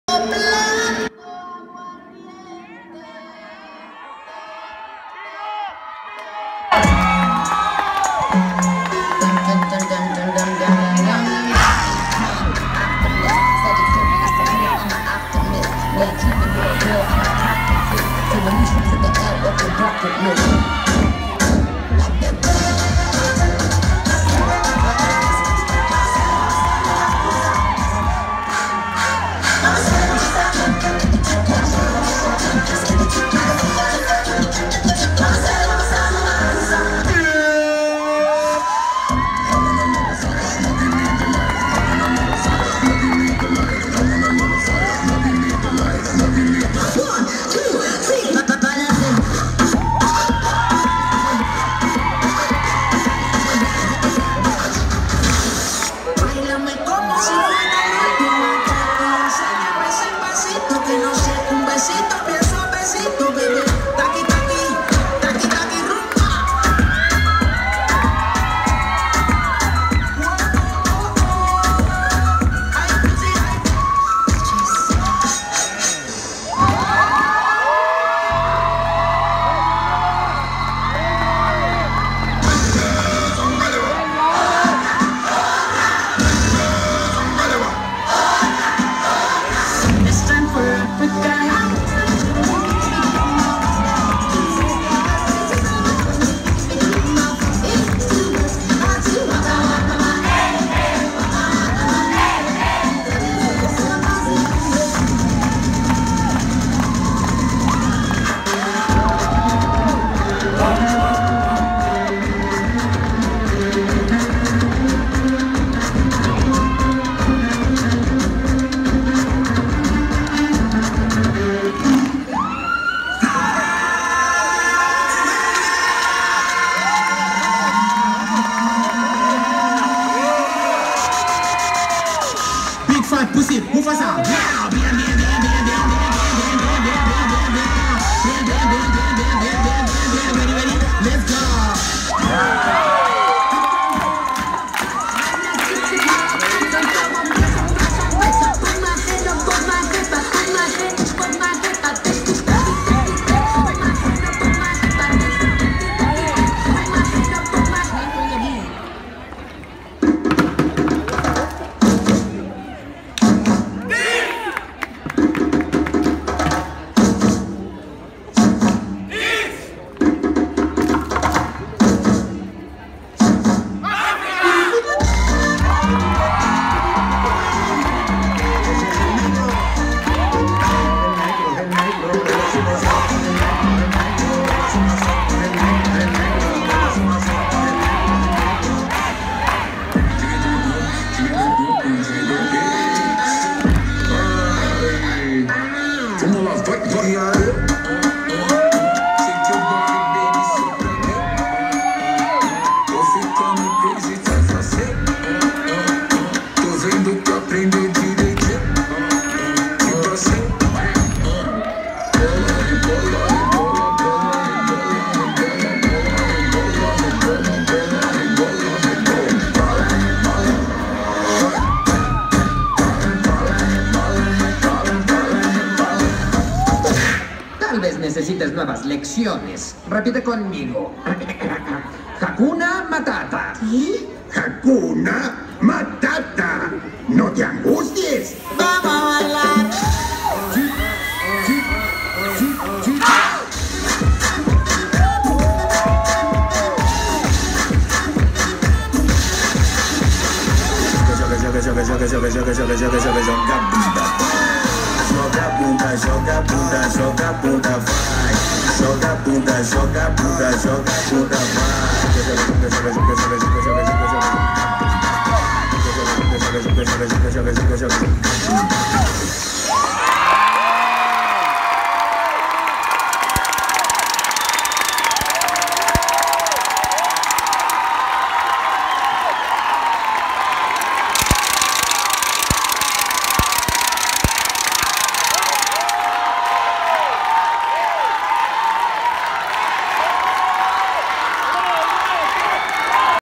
ÁFRICA | INTERCOLEGIAL DE BAILE | UNIVERSIDAD ESPAÑOL ACAPULCO
Esta coreografía representa la cultura de África.